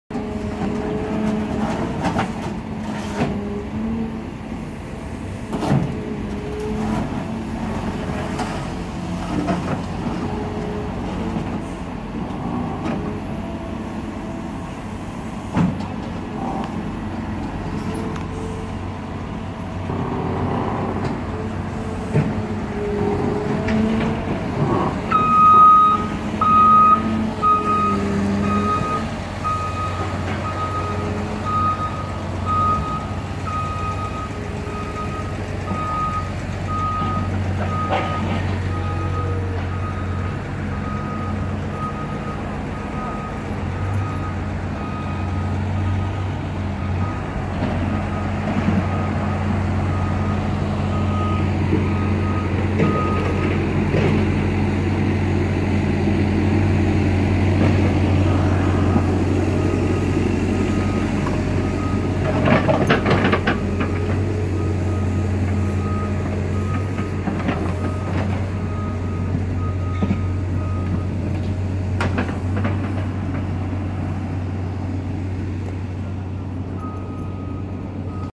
ארלוזורוב תל אביב, הקו הסגול, הריסת שבילי אופניים.
הטמטום חוגג. לפני כשנה סללו שבילי אופניים. באה הרכבת הקלה והנה אין שבילי אופניים. חלם זה כאן.